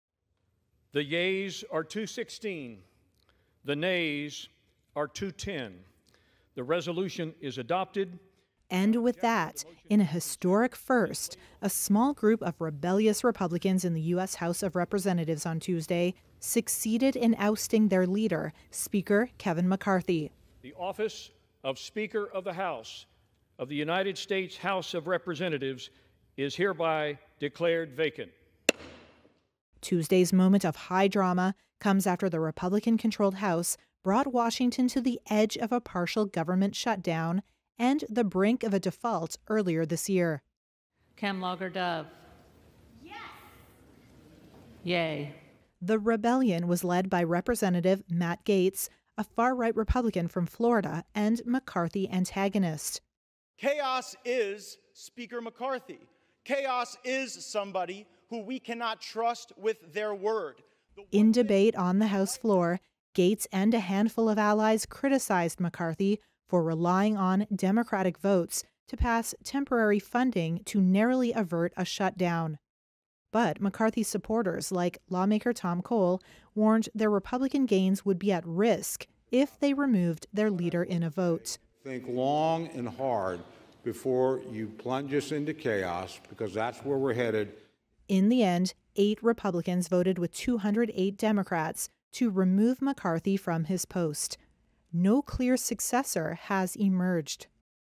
米下院議長を解任、史上初 共和党内紛で議会の混乱さらに深まる
米下院は３日、共和党保守強硬派議員が提出したマッカーシー議長（共和）の解任動議を２１６対２１０の賛成多数で可決した。下院議長の解任は史上初めて。議会は政府機関閉鎖を土壇場で回避したばかりだが、共和党内の内紛でさらなる混乱に陥った。